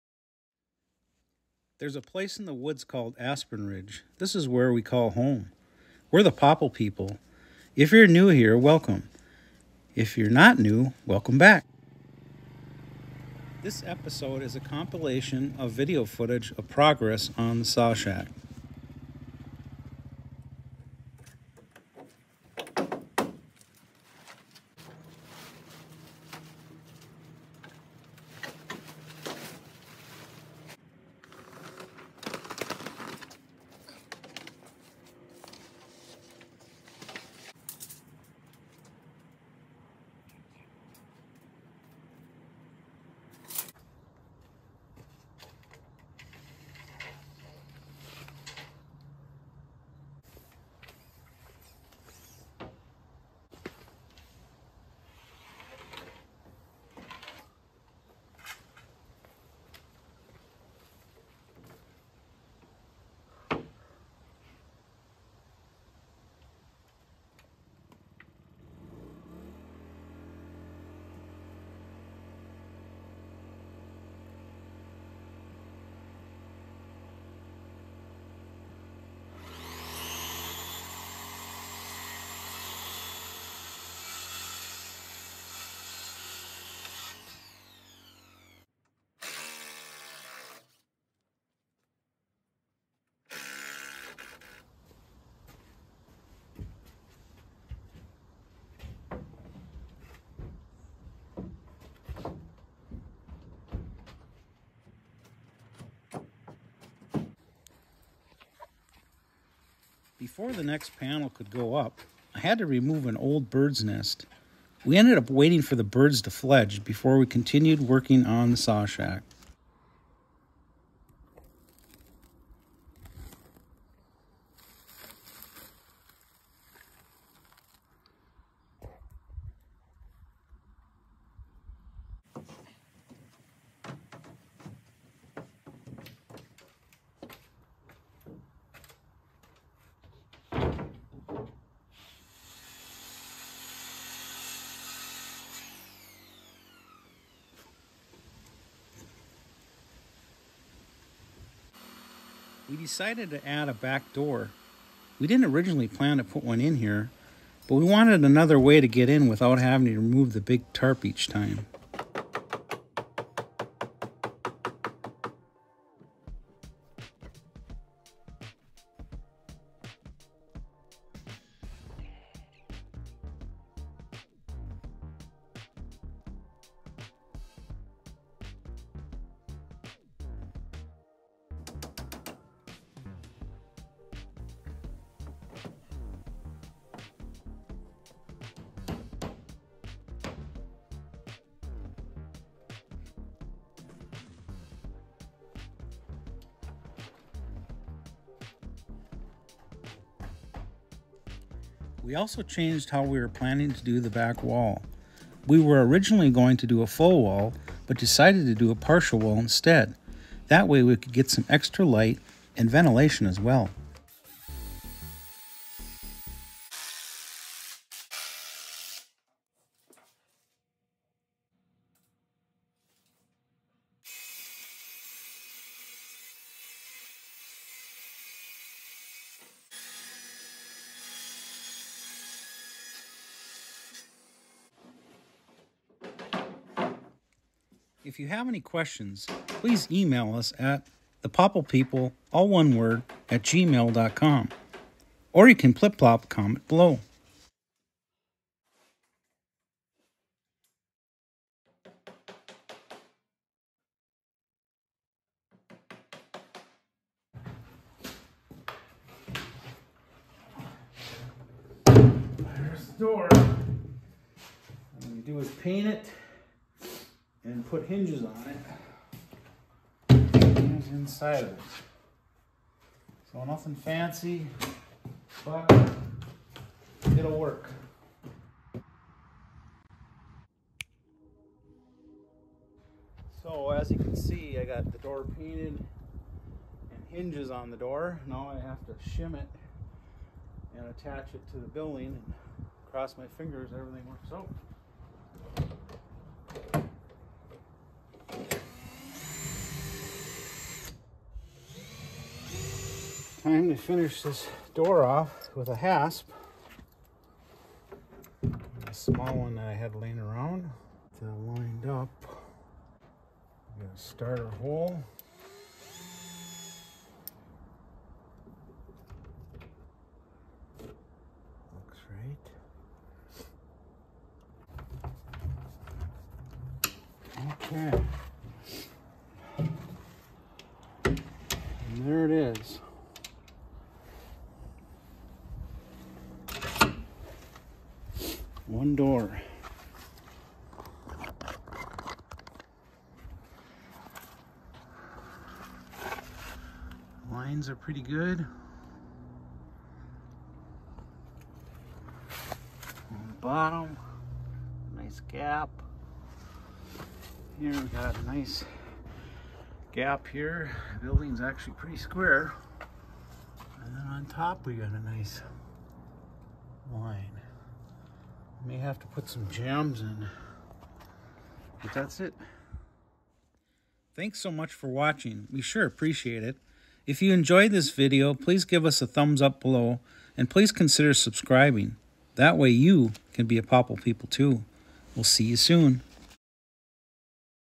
SAW SHACK UPGRADES | Adding a Door, “Real Walls,” More Progress | Popple People | Episode 100!!🎉
In this episode (our 100th “regular” episode!!), we have made some more progress on the Saw Shack! Some of this is video footage from the end of last summer, fall, etc. We are plugging away at it little by little & it’s coming along!